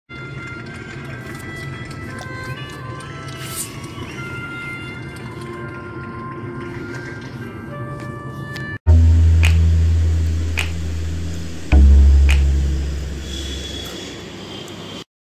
Rabbits Love CarRide | Rabbits in a Park
Rabbits enjoying car ride with soothing music.
A day in the park, rabbits having fun and enjoying the sunshine.